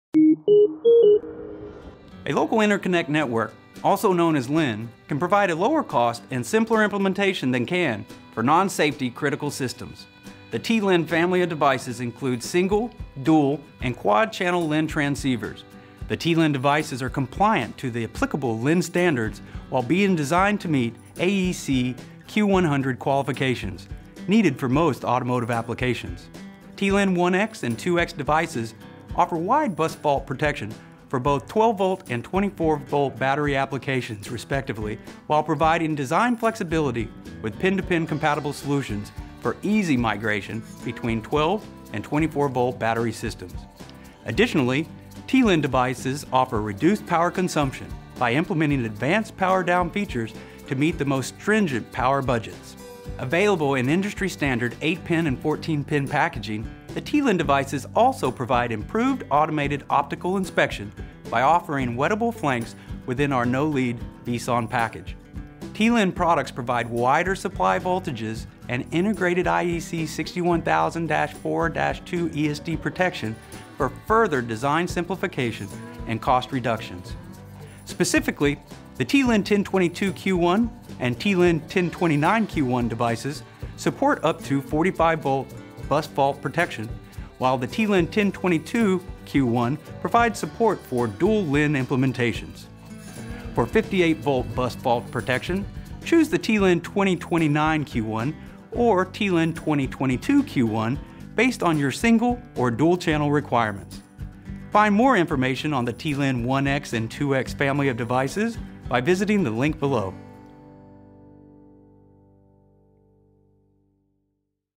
Automotive Interfaces: Local Interconnect Network (LIN) products
View the entire LIN portfolio

Watch this video to learn more about Local Interconnect Network (LIN)
transceivers and how TI's family of TLIN devices meets industry standards and
high performance specs for 12V and 24V applications.

Read this blog to learn more about LIN in 24V applications